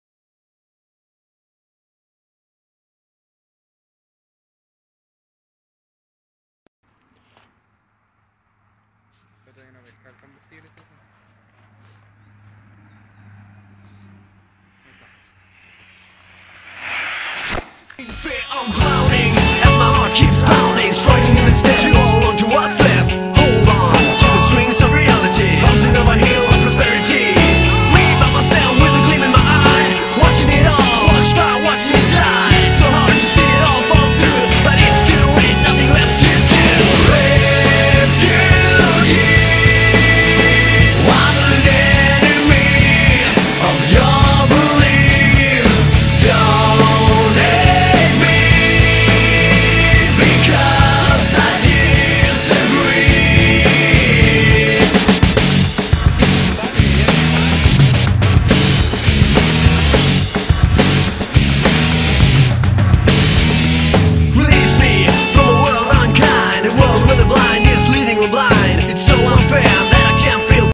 Cohete Combustible Solido (Azucar + KNO3), Chile, Los Angeles xD
La calidad del video esta media mala, mi hermano no sabe grabar bien ¬¬